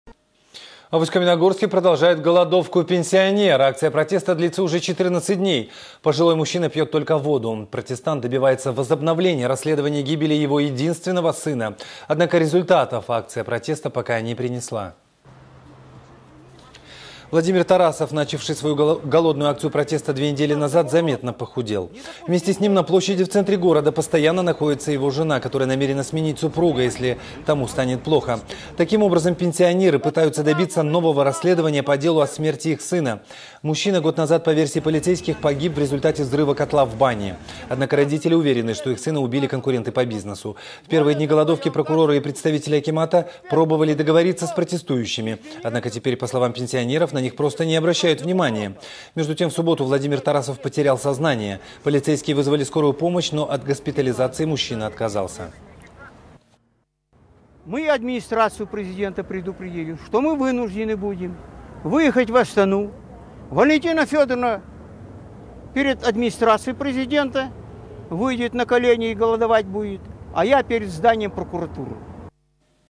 В Усть-Каменогорске продолжает голодовку пенсионер
В Усть-Каменогорске продолжает голодовку пенсионер. Акция протеста длится уже 14 дней. Пожилой мужчина пьет только воду. Протестующий  добивается возобновления расследования гибели его единственного сына. Однако результатов эта акция протеста пока не принесла.